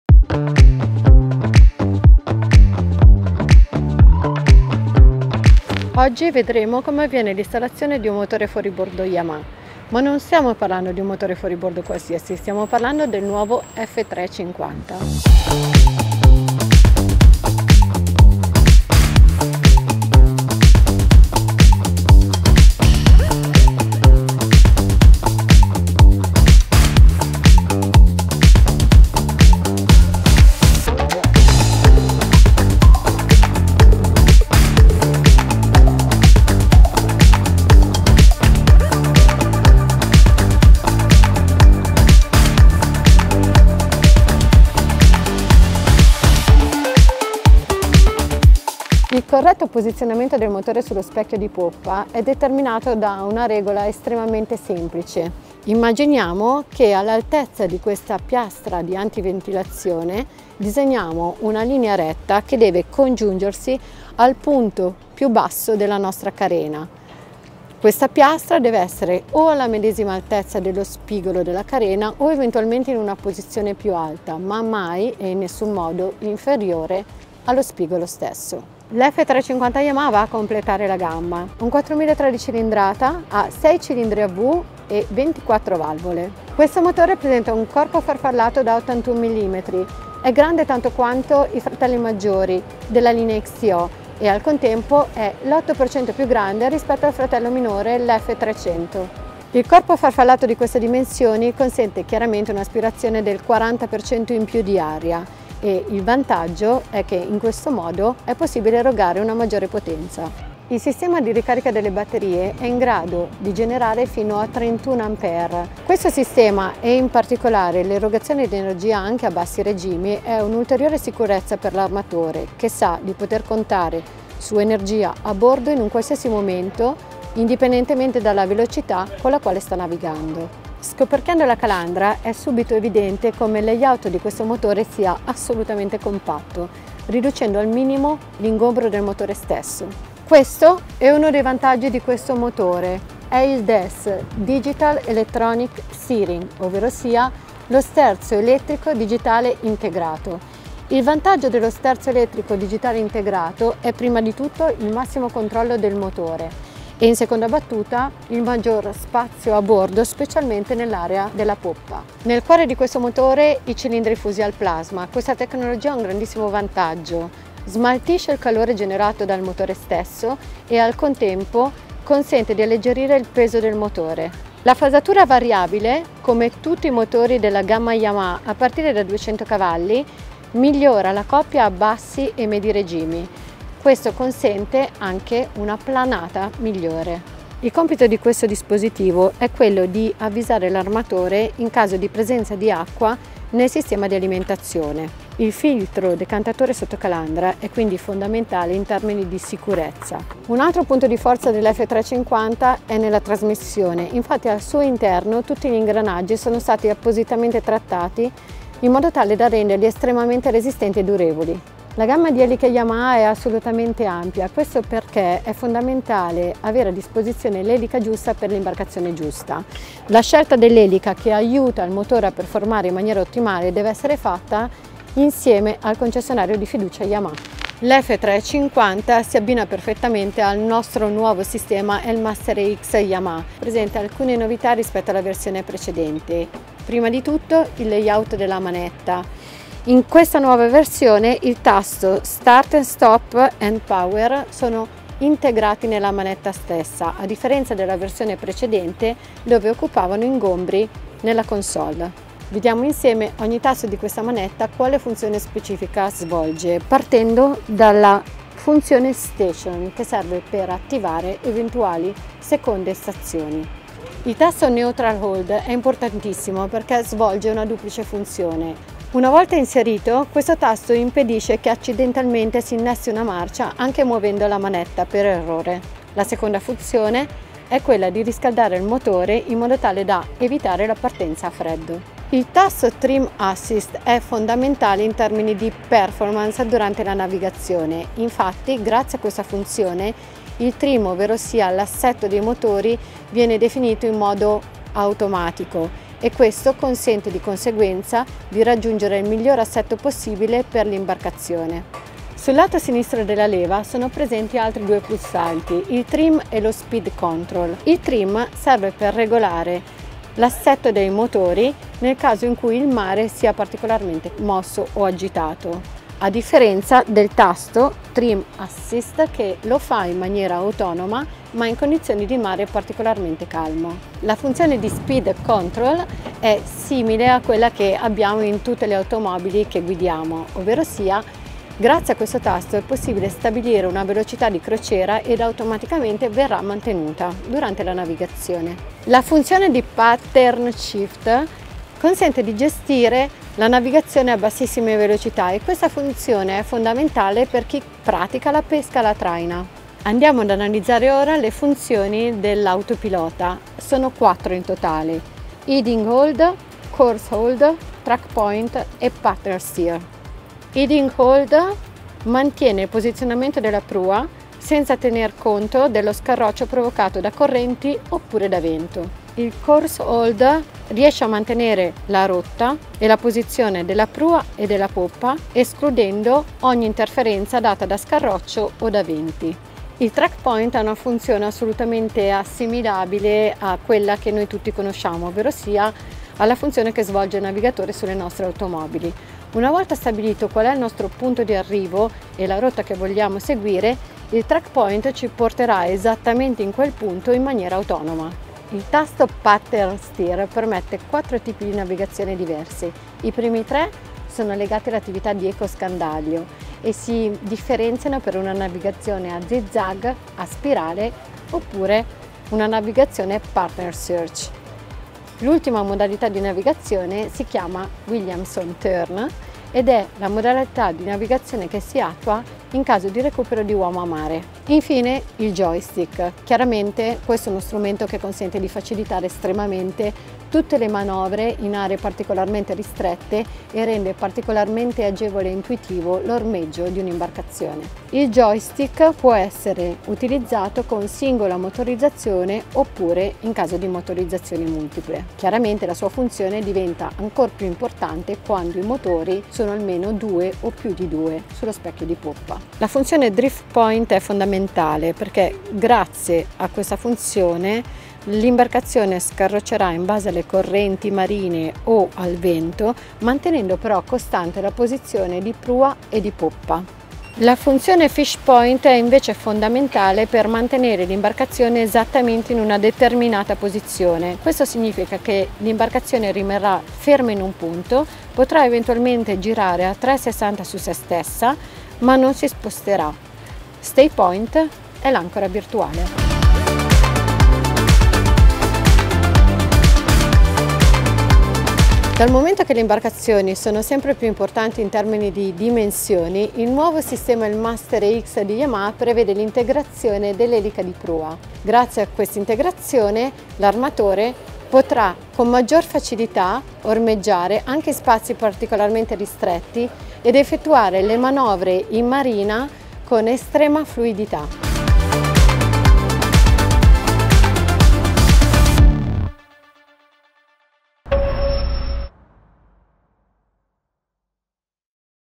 Yamaha 350hp V6: secrets of the outboard with adrenaline character (IT)
In this video we take a detailed look at the 350hp V6 engine, Yamaha's new outboard designed to deliver unparalleled excitement on the water. The 350hp V6 engine is packed with innovative features, including digital electric steering (DES), TotalTilt function, and Thrust Enhancing Reverse Exhaust (TERE), to deliver the best in boating capabilities.